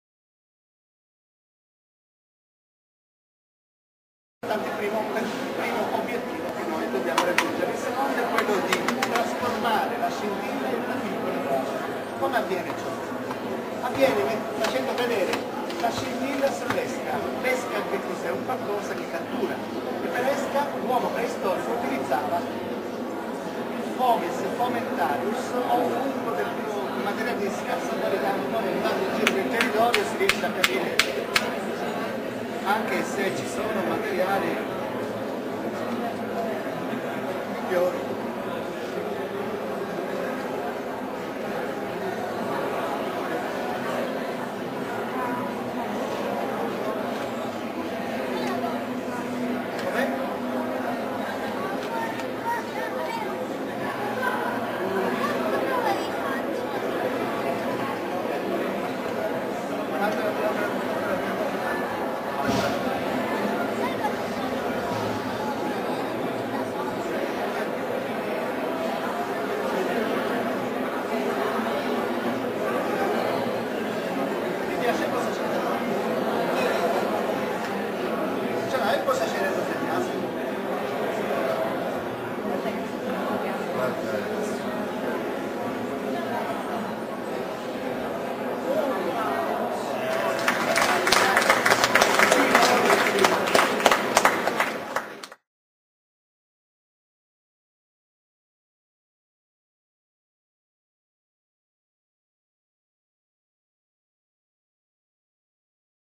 Accensione del fuoco, Paestum 2006
Borsa del Turismo Archeologico di Paestum, anno 2006